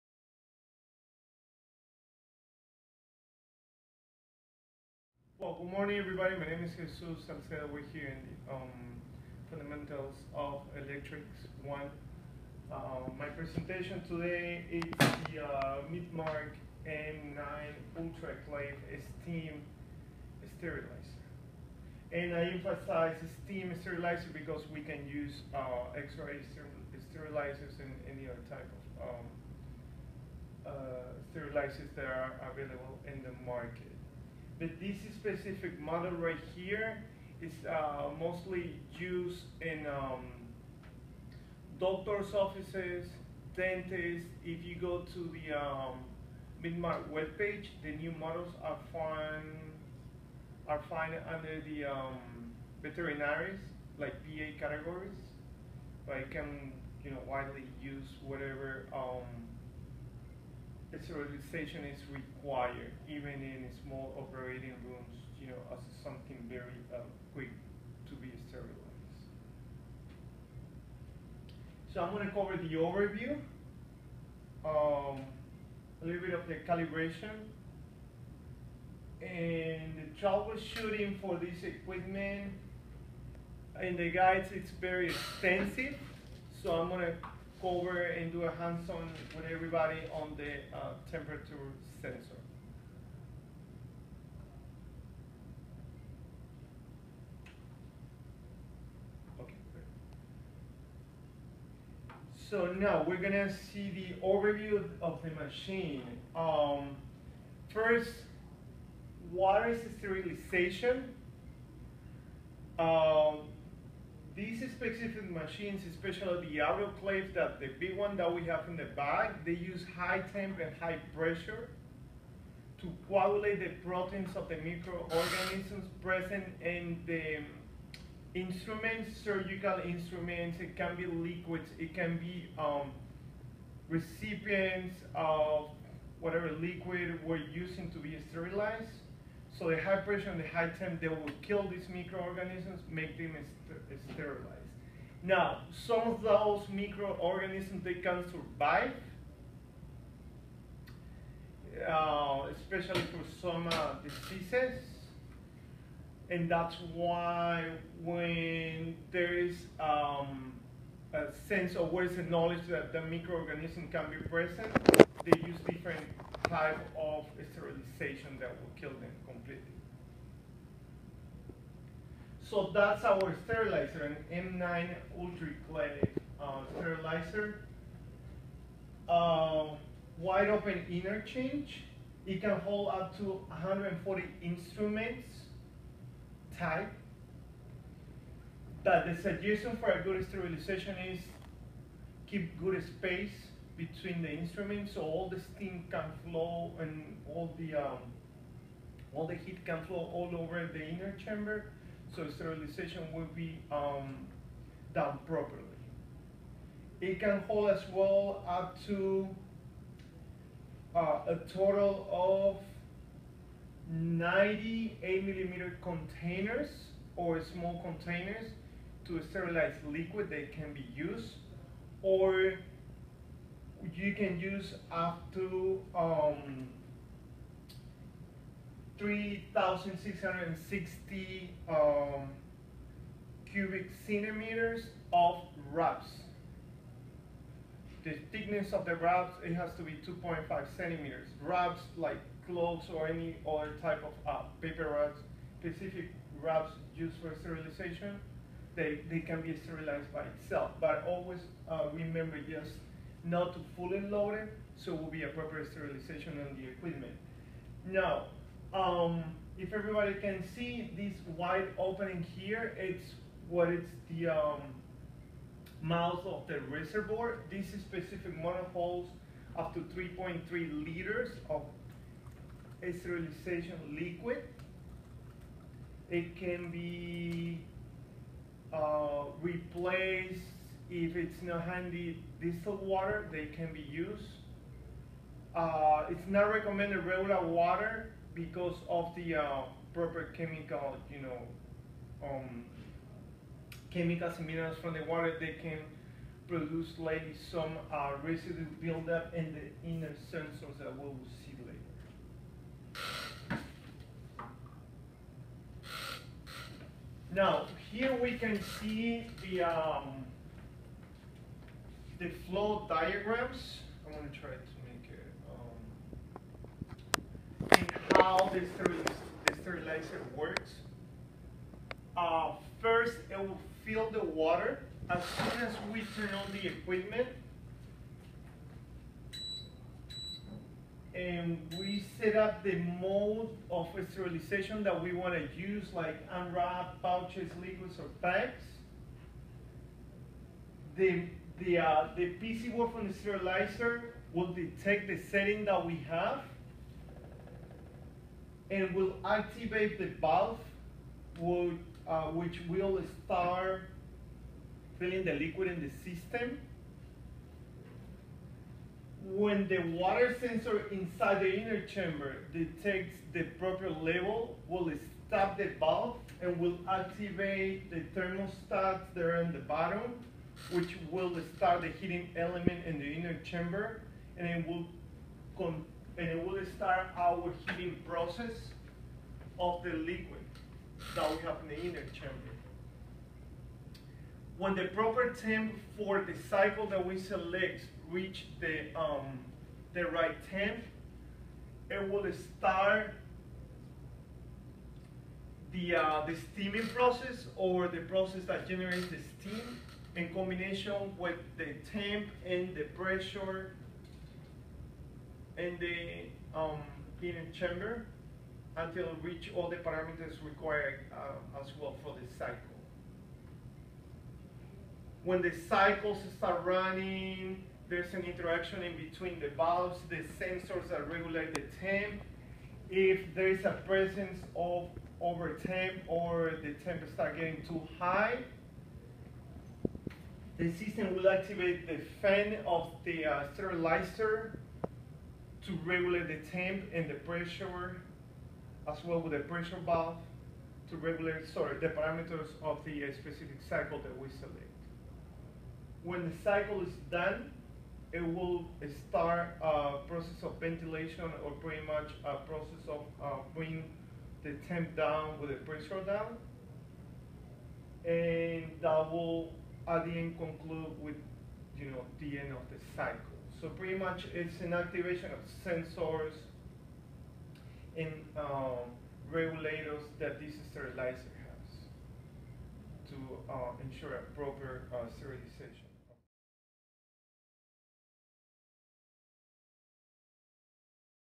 Presentation of a Midmark M9 UltraClave unit.
This video contains a brief presentation of a Midmark M9 UltraClave unit.